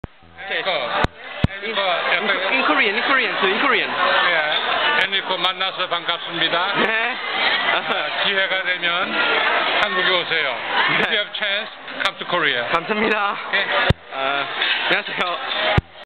Please come to Korea! 한국에 오세요!
It's just a random video. While I was in Greece I met a huge group of Korean people from Sogang Catholic University (서강 대학교) and this man invited me to come to Korea if a get a chance =P. "Enrico 만나서 반갑습니다! 기회가 돼면, 한국에 오세요".